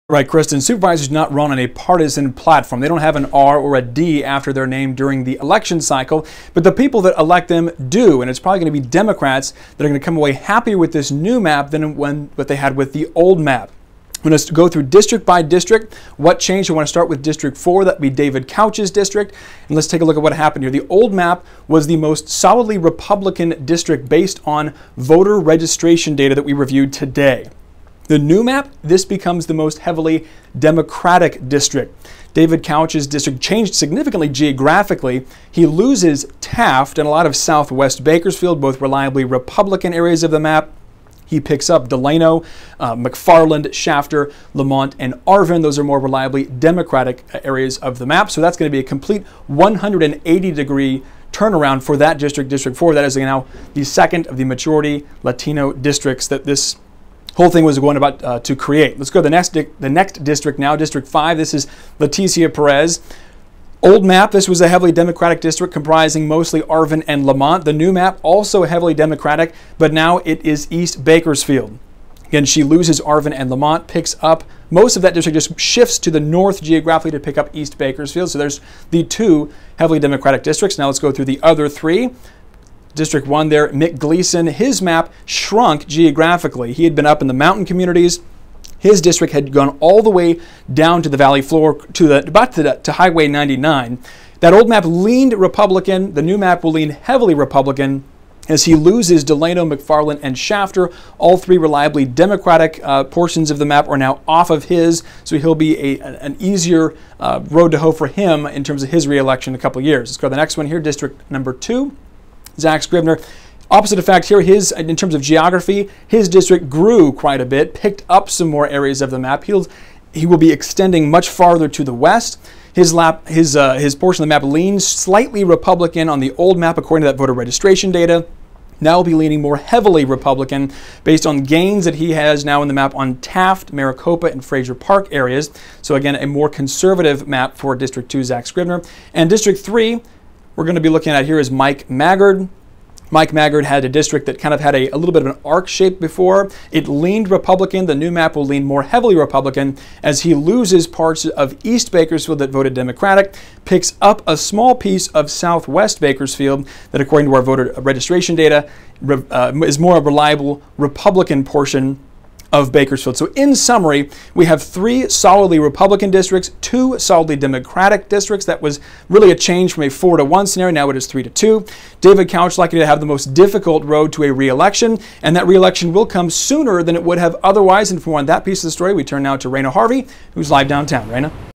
What's the political impact of the supervisors' new district lines?
County supervisors don't run on a partisan platform.